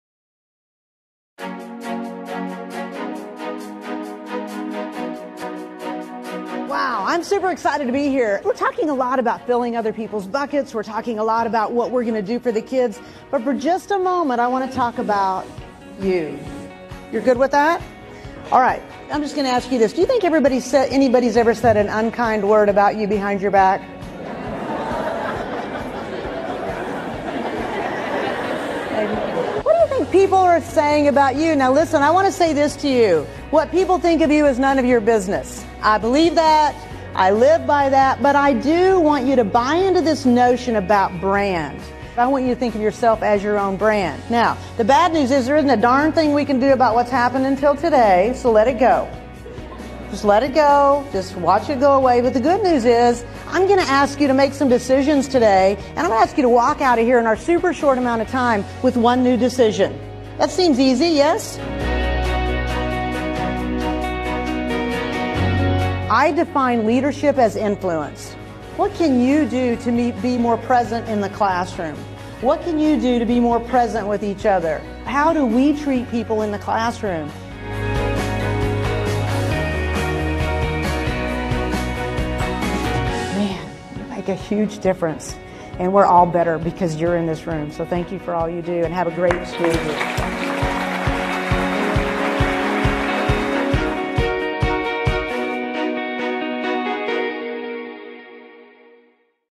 Tracy Spears: "Motivational Presentation to Educators"
Electrify your meeting with this dynamic speaker and co-author of the best-selling books "What Exceptional Leaders Know" and "Exceptional Leaders Playbook." She will entertain and inspire your group. Hire Tracy to speak at your next event.